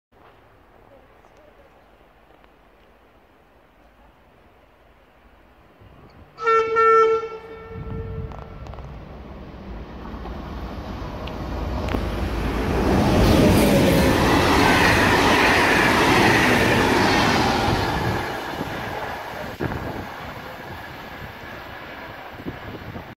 TCS Dinner Train komt met tyfoon door station Tilburg Reeshof 23-10-2021. Dutch train
NL: TCS Dinner Train komt met tyfoon door station Tilburg Reeshof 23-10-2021. Veel kijkplezier

UK: TCS Dinner Train comes with typhoon through Tilburg Reeshof station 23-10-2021. Enjoy watching